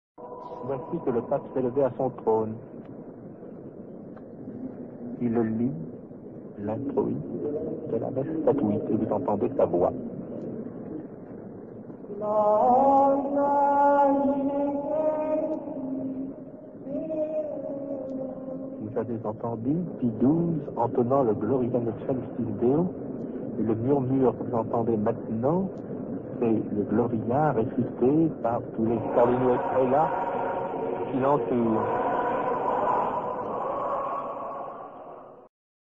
(French) PTT Reportage - De Lintronisation Du Pape PIE XII - March 12, 1939
PTT News report from March 12th 1939 in French language.

Audio and Image are Public Domain (Copyright, Designs and Patents Act 1988)